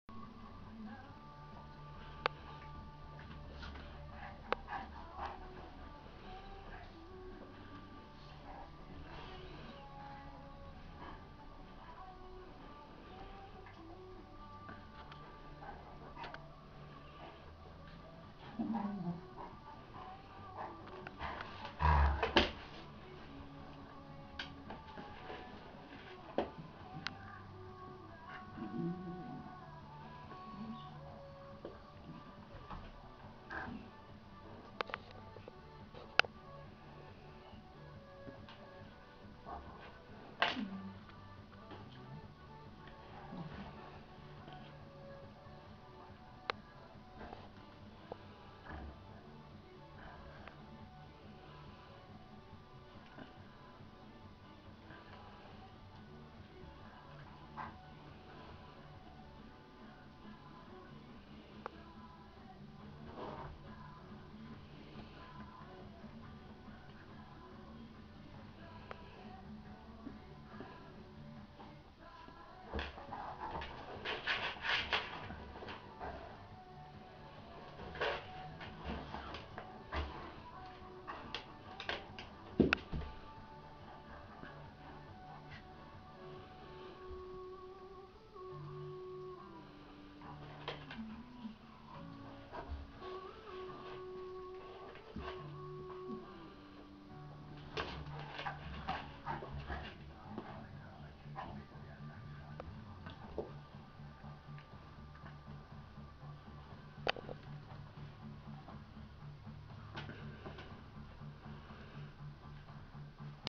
Irish Wolfhound from Starstone
birdy & lucy
Morgentliche Wäsche der Ohren von Cocker Lucy !